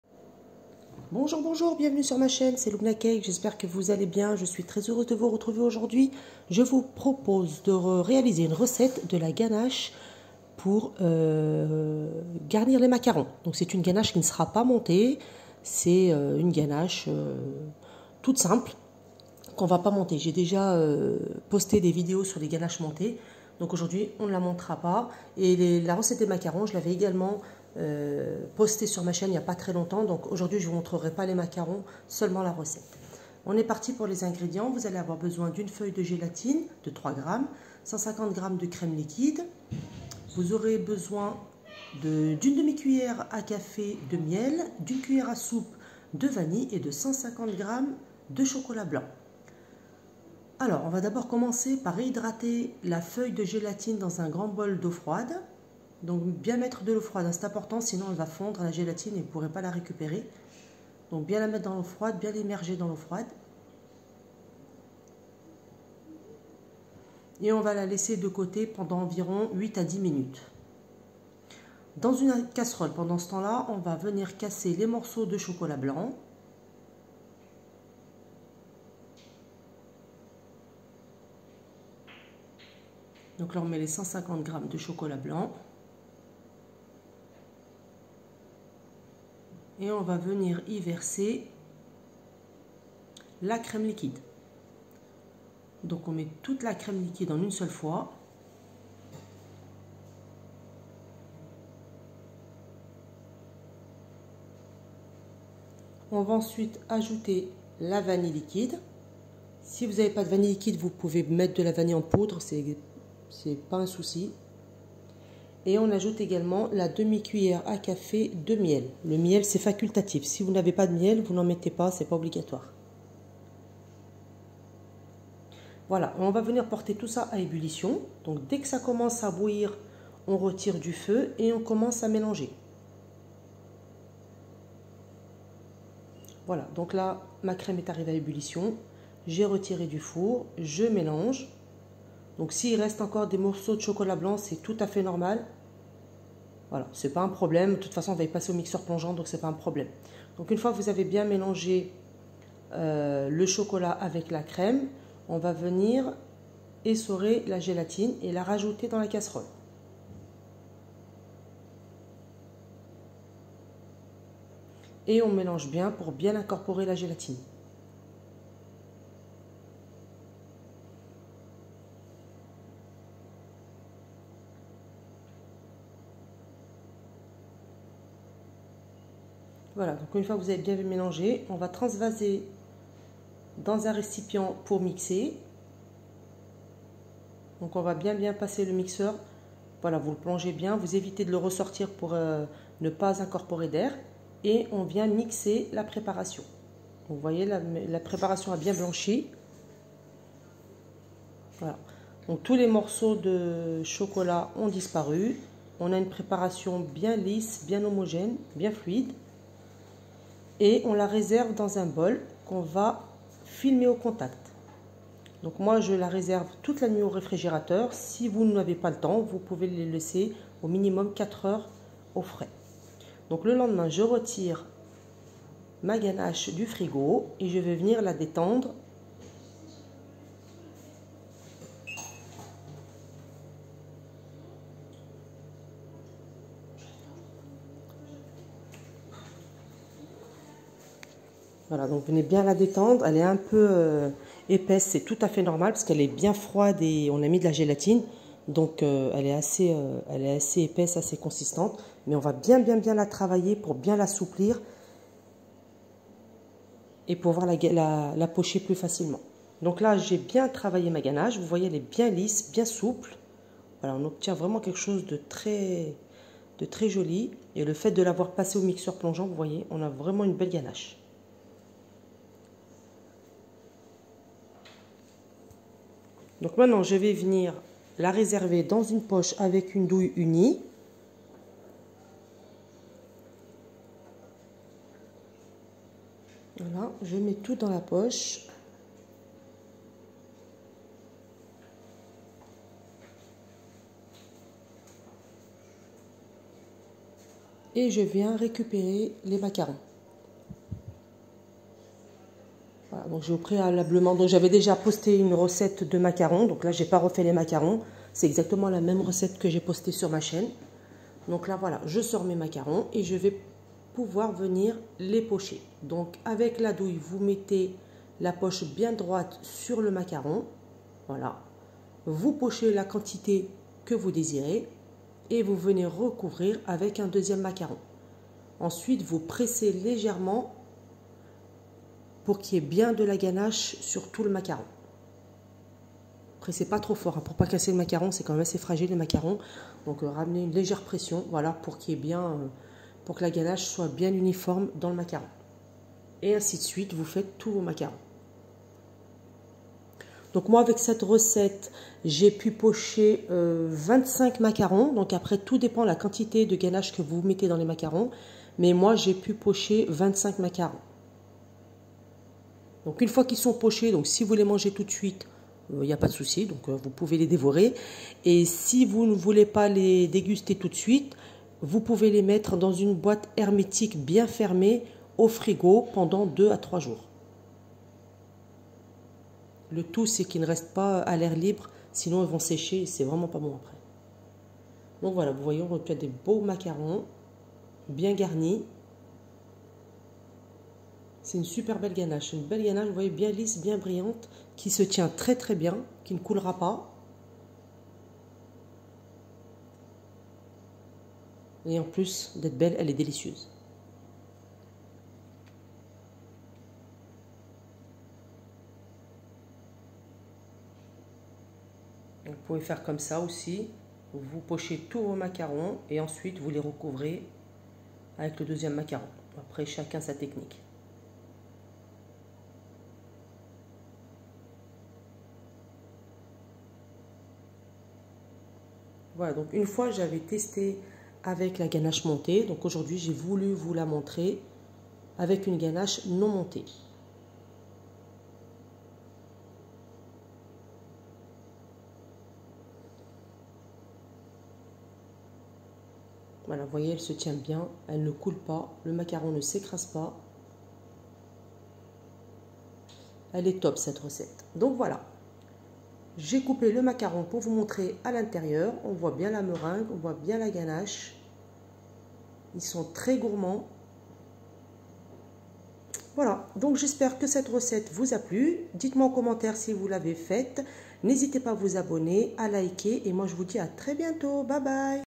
Ganache chocolat blanc pour macarons.
coucou je vous poste aujourd'hui la recette de la ganache non. montée au chocolat blanc pour garnir vos macarons.

ingrédients :

150 g de chocolat blanc
150 g de crème liquide à 30% min de mat. gros
1 feuille de gélatine ( 3g)
1/2 c à c de miel ( facultatif)
1 c a s de vanille liquide ou en poudre

faite fondre la gélatine 8 à 10 min dans un grand bol d'eau froide, mettez le reste des ingrédients dans une casserole, portez à ébullition mélangez bien et ajouter la gélatine essorée

si il reste des morceaux de chocolat qui n'ont pas fondus ce n'est pas grave car on va passer la ganache au mixeur plongeant.

Mixer le tout au mixeur plongeant et réserver dans  un bol filmé au contact toute une nuit ou minimum 4h.
le lendemain mélanger la ganache fortement jusqu'à ce que la préparation devienne bien souple et lisse.
mettez da's une poche avec une douillé unie ou cannelé et garnissez vos macarons.

voilà 😊😊😊😊 bon app à tous.

dite moi en  commentaires si vous avez essayé la recette et si elle vous à plu.

𝙂𝙍𝙊𝙎 𝘽𝙄𝙎𝙊𝙐𝙎 À 𝙏𝙍È𝙎 𝙑𝙄𝙏𝙀.
𝙇𝙊𝙐𝘽𝙉𝘼_𝘾𝘼𝙆𝙀